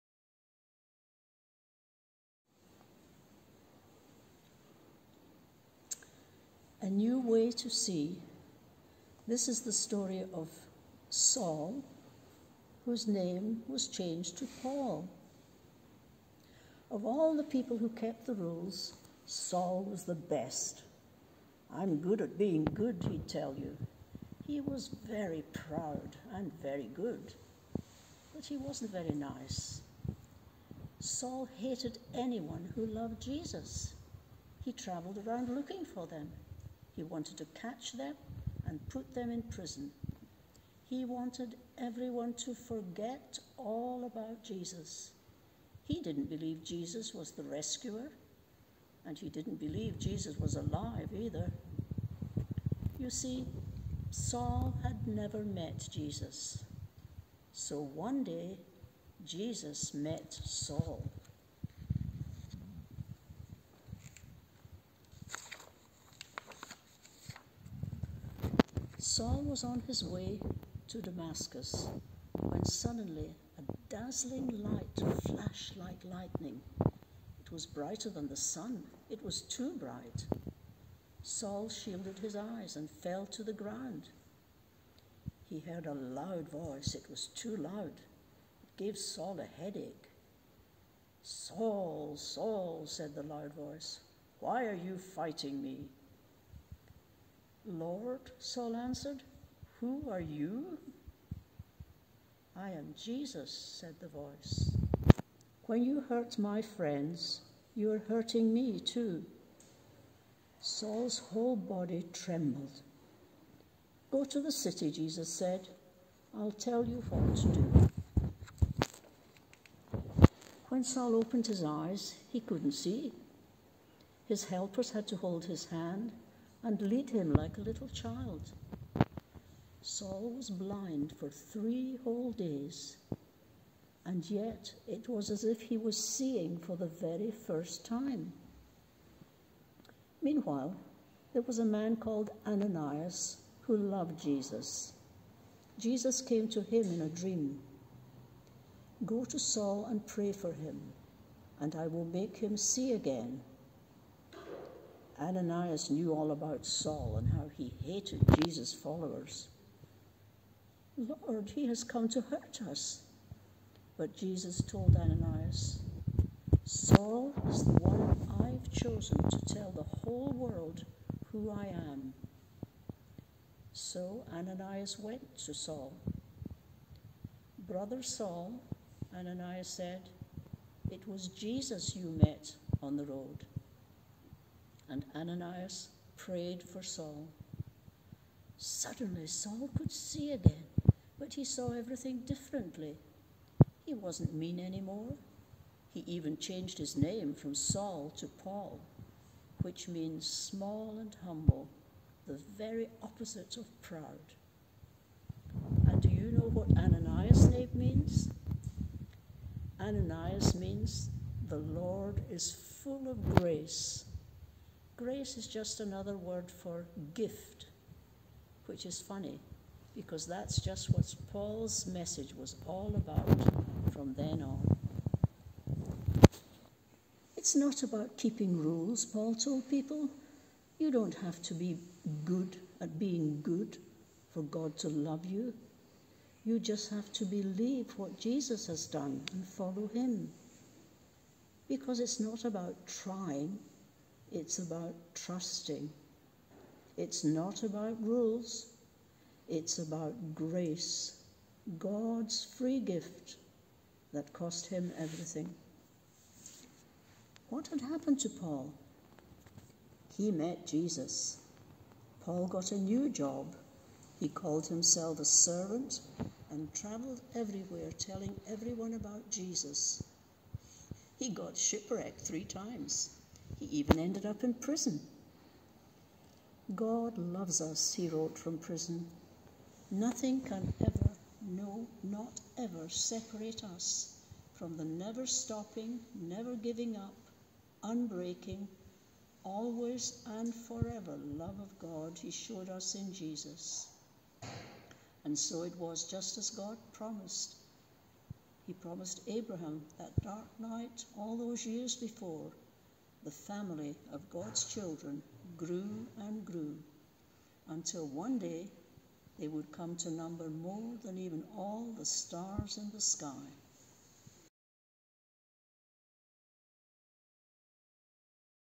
Jesse Tree No. 29 Paul
Did you know that the Christmas story is part of a bigger story?

If you would like to read this story about about Paul in the NIV translation in the Bible, read Acts 9:1-19

Can you guess what item might have been made for today's story and hung up on the giant Jesse Tree outside St Peter's? Can you spot the item on the tree when you walk past? If you're making your own Jesse Tree at home, will you make the same item or choose something else?